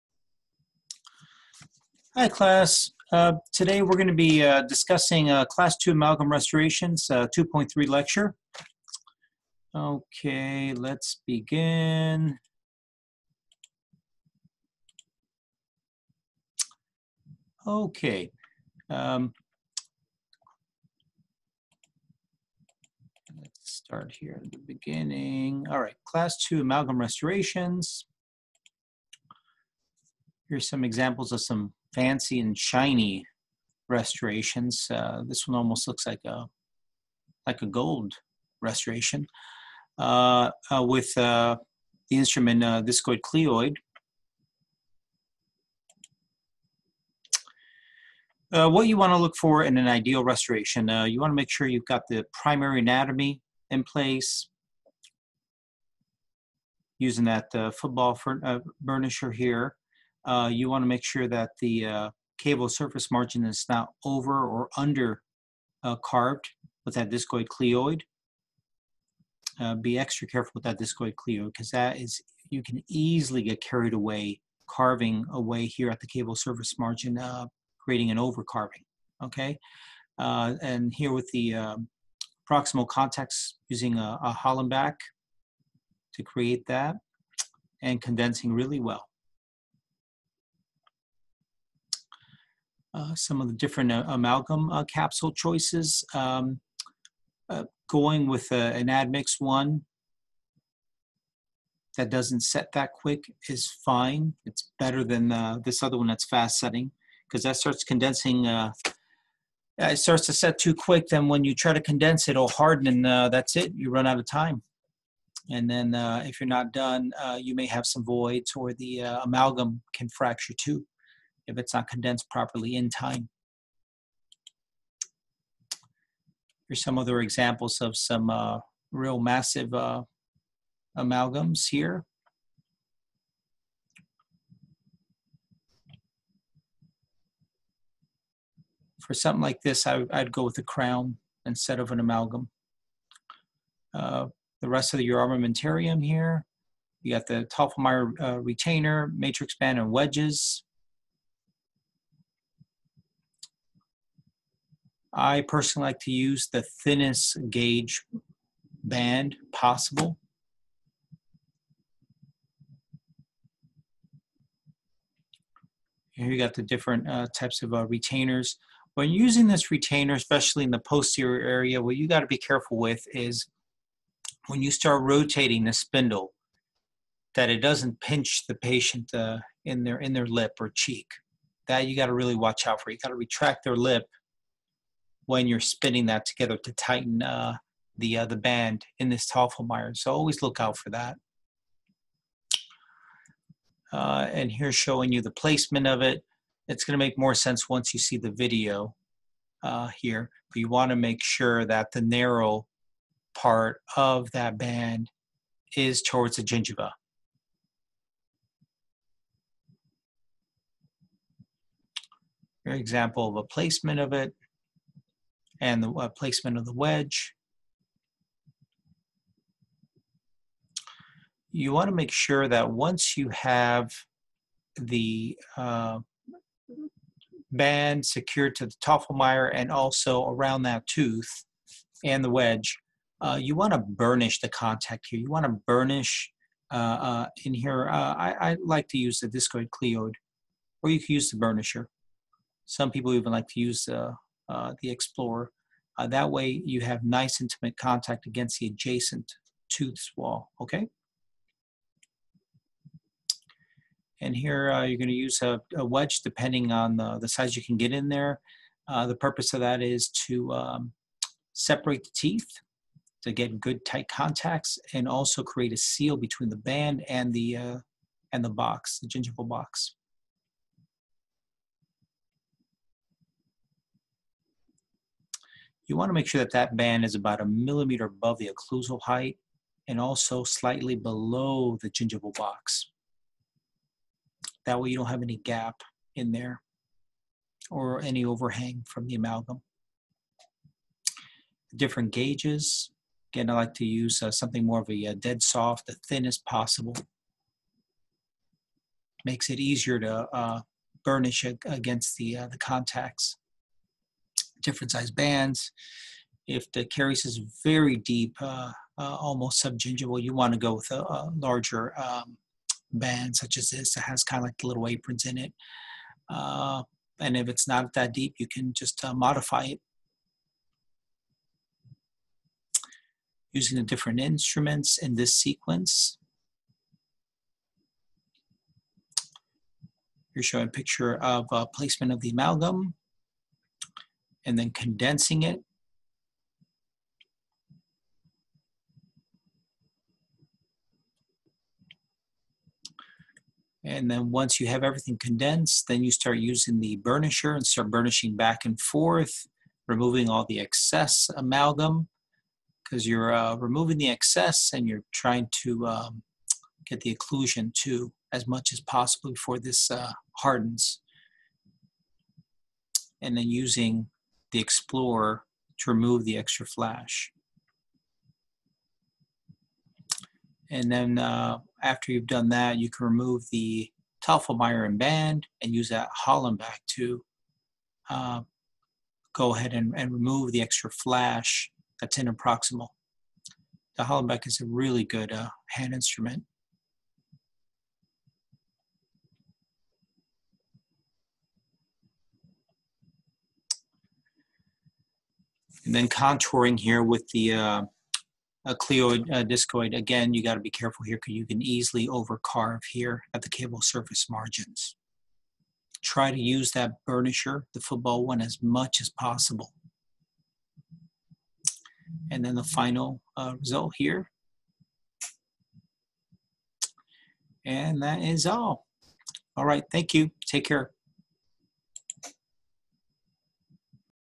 ORDEMY- Cl II Amalgam Restorations (Lecture 2.3)(7 mins 0.4 secs)
Topics Covered: Cl II Posterior MOD Amalgam
1. Amalgam Types
2. Matrix Bands and Wedging technique
3. Carving and re-creating anatomy with amalgam
4. Finishing